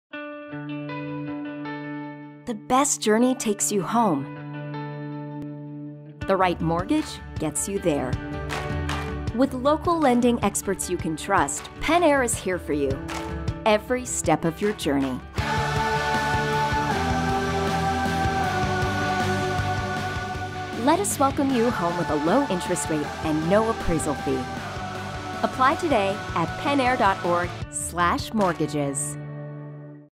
Pen Air Federal Credit Union
Welcome Home. The best journey takes you home. The right mortgage loan gets you there.  We'll pay for the appraisal on a new home purchase. Most of our loans close up to 15 days quicker than other lenders.